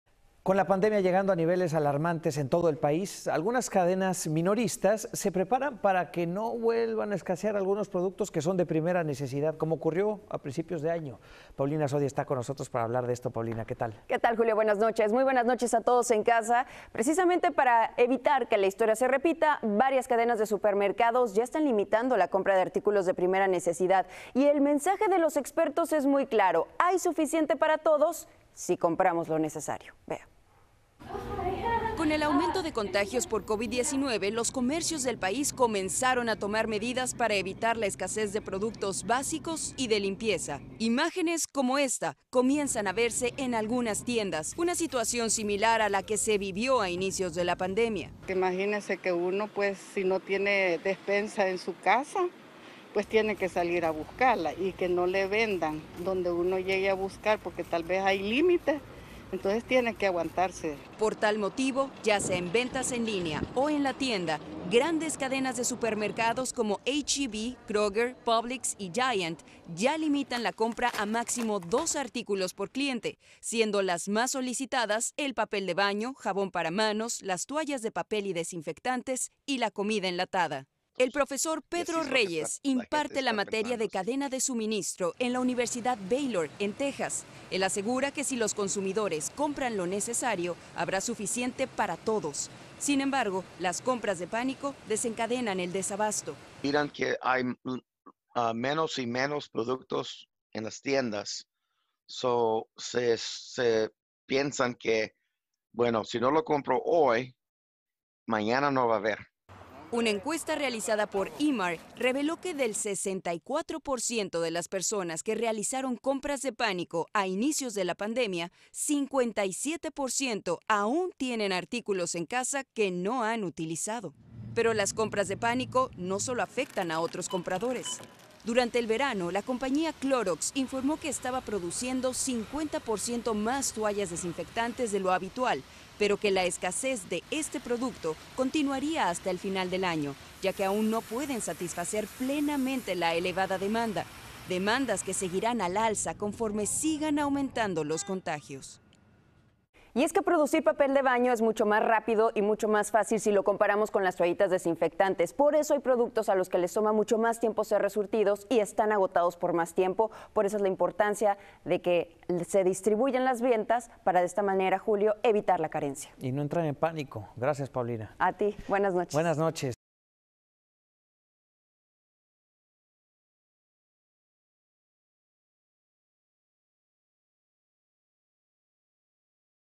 Toman medidas para evitar la escasez de productos básicos | Noticias Telemundo
Video oficial de Noticias Telemundo. Ante el aumento de los contagios de coronavirus en todo el país, los dueños de negocios han notado un incremento de las compras, por lo que están tomando medidas para evitar que se repitan los incidentes del primer brote.